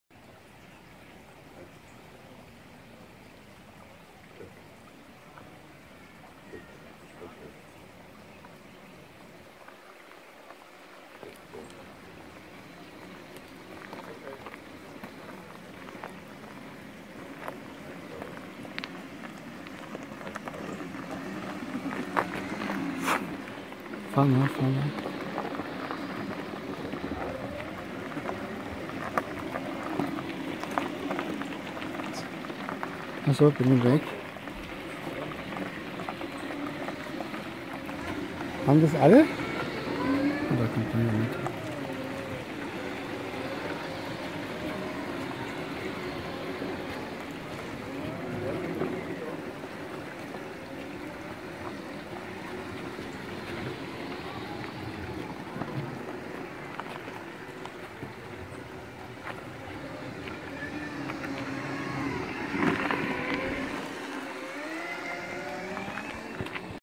Siebenmühlenal mit dem Segway
Vom Bundeswanderweg zur Kochenmühle - aufregende Abfahrt